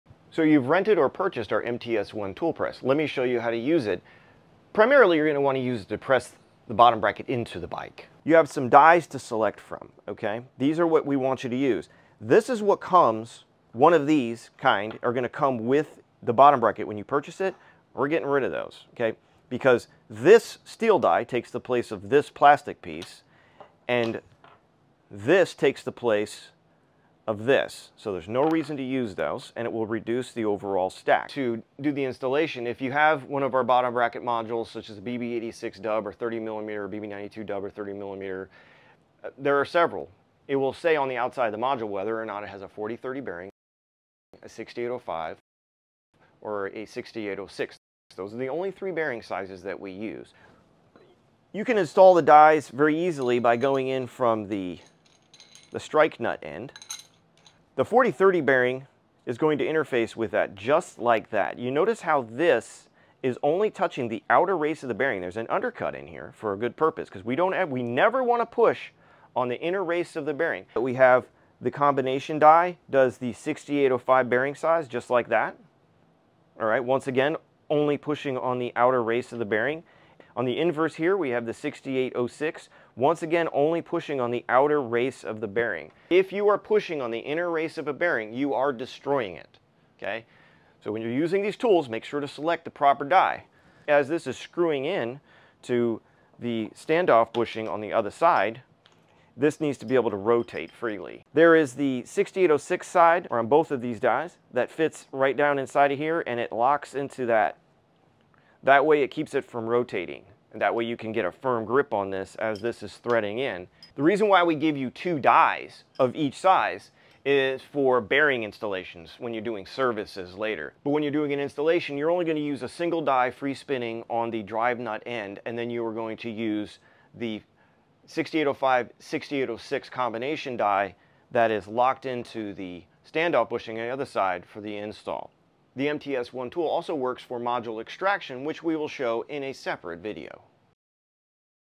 BBInfinite MTS-1 Tool User Guide: Setup for installation
In this instructional video we will show you how to setup our MTS-1 Tool Press for a press fit bottom bracket installation.